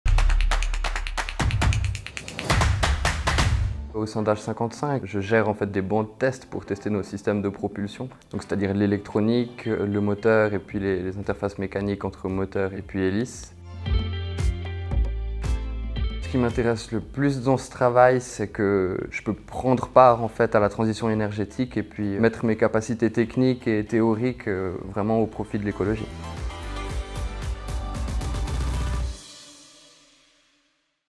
Bachelor of Science (BSc) HES-SO ingénieur·e en Systèmes industriels
Cette formation en ingénierie multidisciplinaire couvre un large spectre technologique orienté vers
l’innovation industrielle - de l’électronique à la digitalisation, de l’automatisation à la robotique, de l’impression 3D à la conception mécanique. Afin d’apprendre à créer des systèmes industriels connectés et plus durables, trois orientations sont proposées : Infotronics, Power&Control,
Design&Material.

School of Engineering - Valais - Wallis - Sion - HEI - Haute Ecole d'Ingénierie
Institut Technologies du vivant - Campus Energypolis